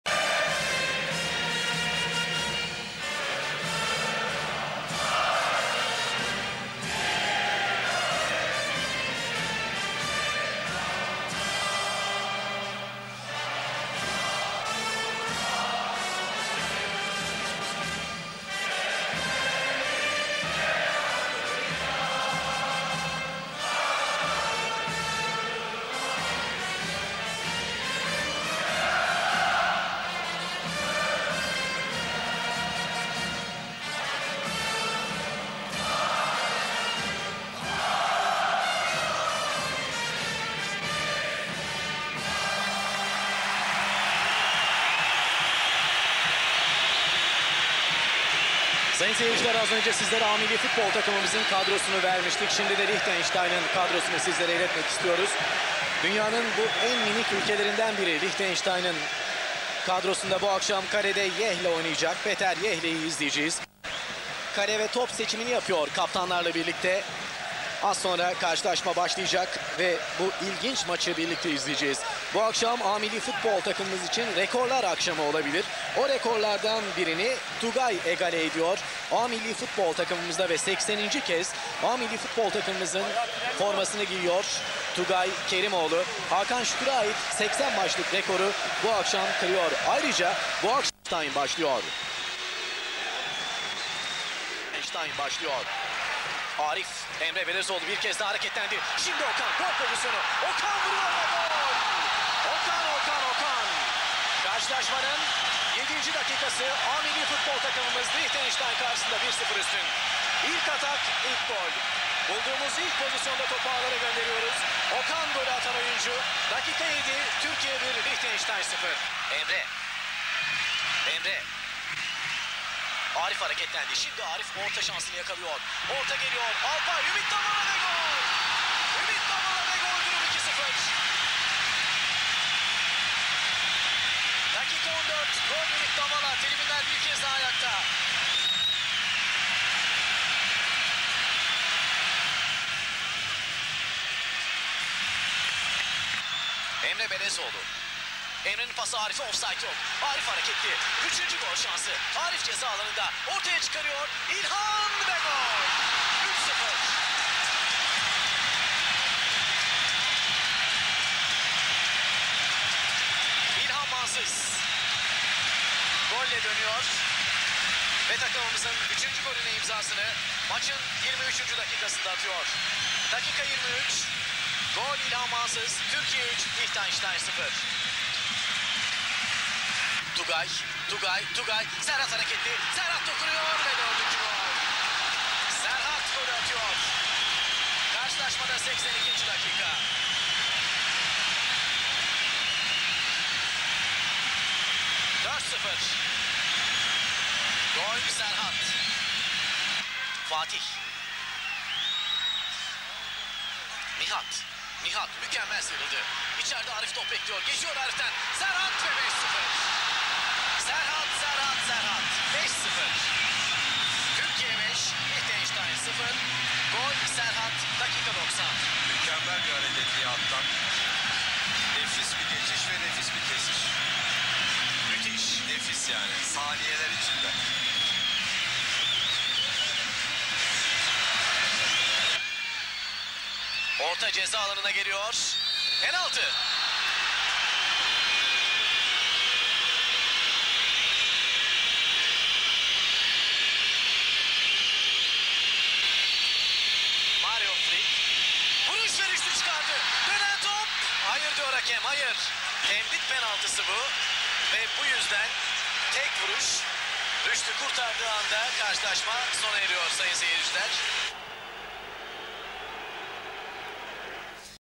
Türkiye 5-0 Liechtenstein (16.10.2002)
Euro 2004 Elemelerinde Millilerimiz, Ali Sami Yen'de Liechtenstein'i 5-0 yenerek gruba 3'te 3 ile başlıyor.

Stad: Ali Sami Yen
Hakem: Yuriy Baskakov (Rusya)
Goller: Okan Buruk (7'), Ümit Davala (14'), İlhan Mansız (23'), Serhat Akın (81' ve 90')

Türkiye İlk 11:
Rüştü Reçber
Bülent Korkmaz
Ümit Davala
Alpay Özalan
Ergün Penbe
Emre Belözoğlu
Okan Buruk
Tugay Kerimoğlu
Arif Erdem
Nihat Kahveci
İlhan Mansız
T.D.: Şenol Güneş

Liechtenstein İlk 11:
Peter Jehle
Fabio D'Elia
Daniel Hasler
Michael Stocklasa
Martin Stocklasa
Martin Telser
Thomas Nigg
Ronny Büchel
Andreas Gerster
Thomas Beck
Mario Frick
T.D.: Ralf Loose (Almanya)